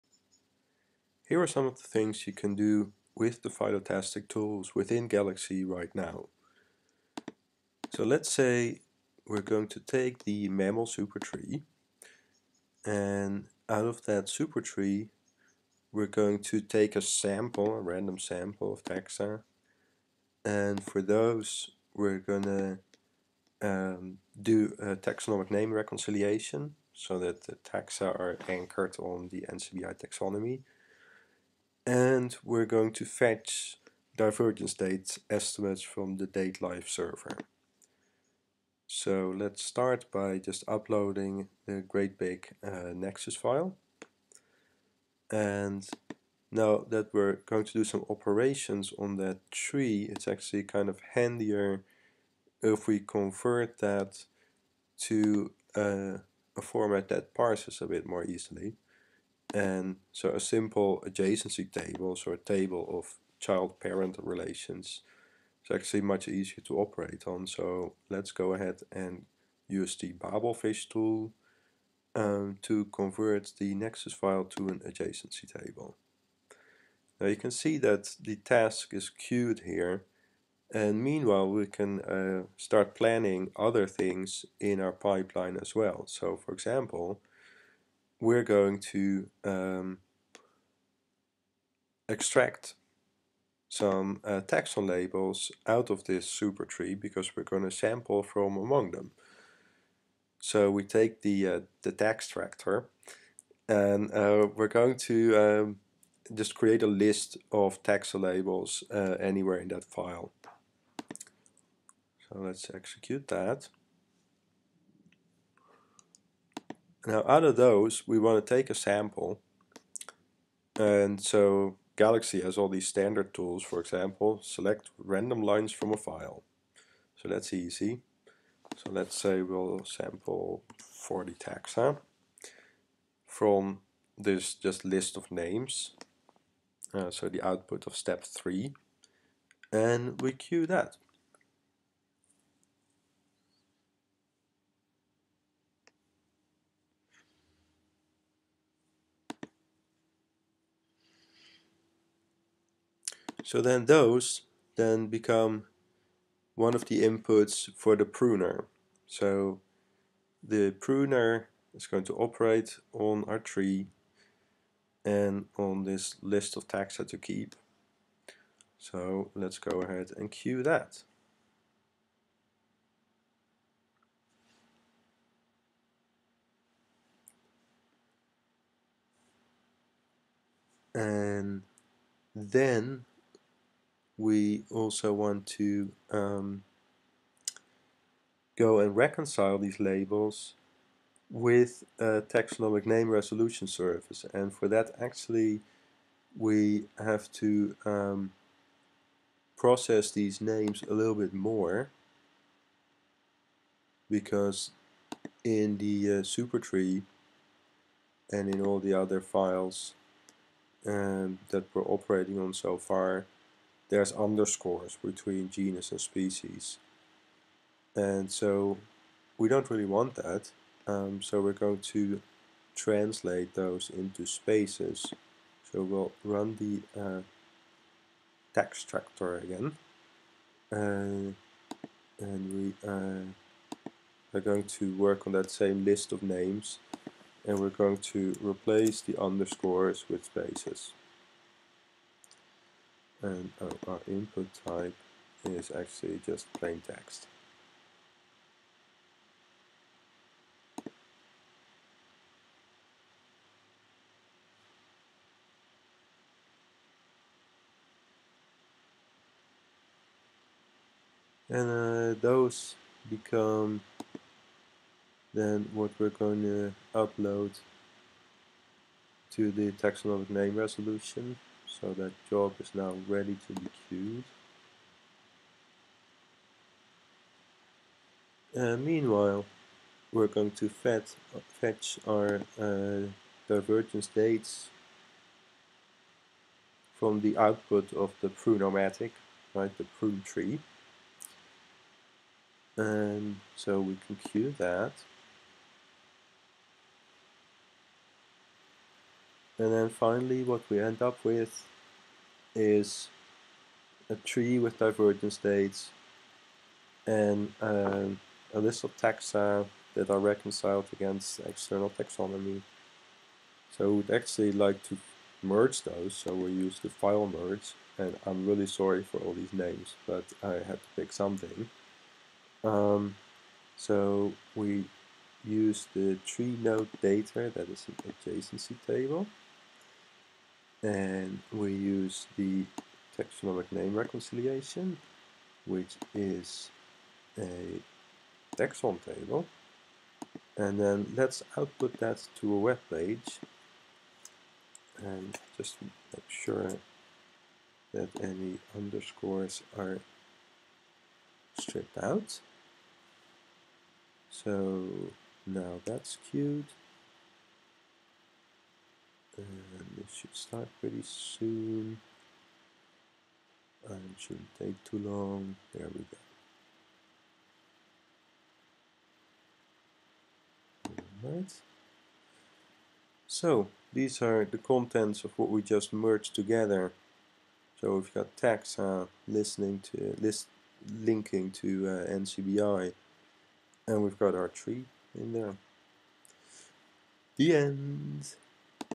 Using PhyloTastic Tools inside Galaxy
An example workflow that samples 40 taxa from the mammal supertree, reconciles their names with the NCBI taxonomy, and retrieves their divergence dates from DateLife.org. Code under development